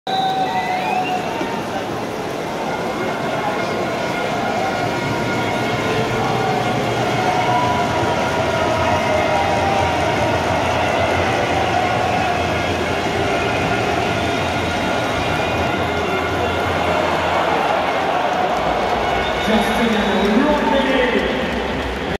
ELF Finale 2021 - 3.Quarter
Frankfurt Galaxy vs Hamburg Sea Devils

Blick vom Oberrang der Gegentribüne.